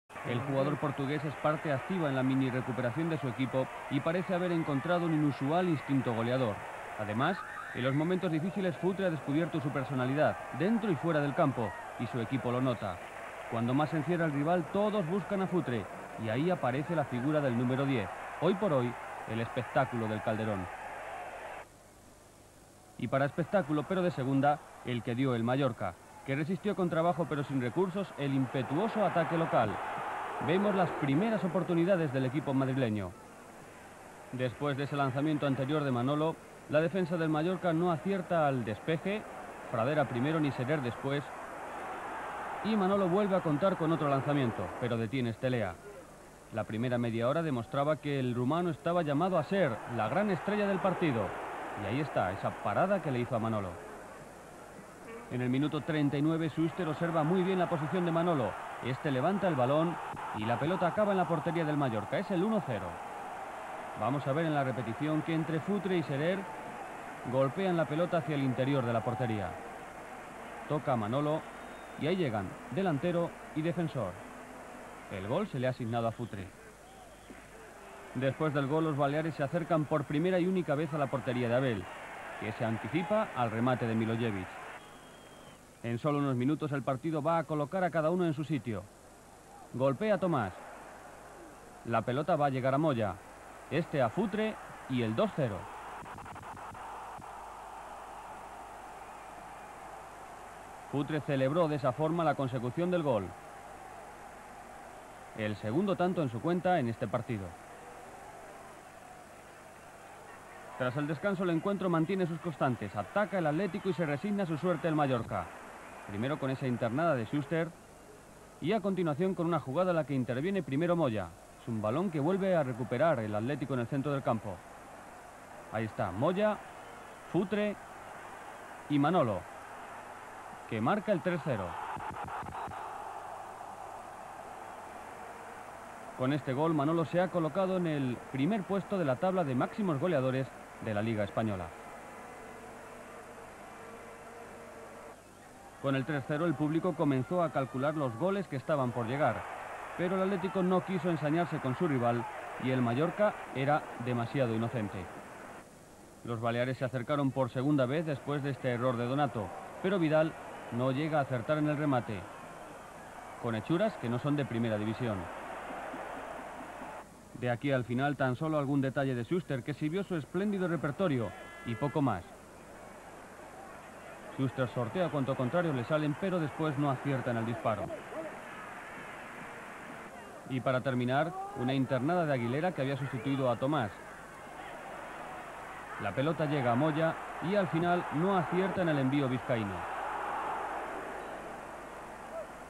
1991-92 At. Madrid 3 - Mallorca 0
Partido de liga de la temporada 1991-92 disputado por el Atlético de Madrid y el Mallorca. Victoria del Atleti por 3 a 0, con tantos de Futre (2) y Manolo.